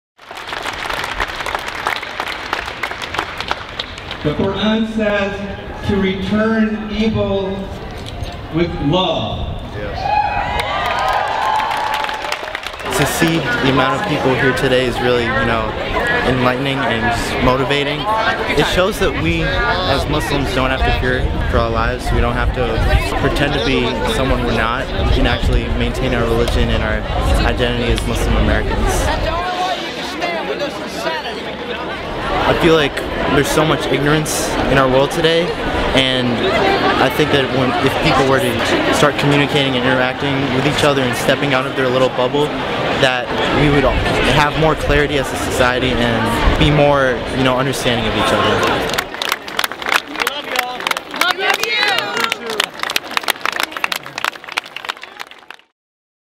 Muslim Day at Texas Capitol draws huge crowd of supporters
In an enthusiastic show of support for Texas Muslims, several thousand people linked arms to cheer speakers and deter protesters at this year’s Muslim Day event at the Capitol.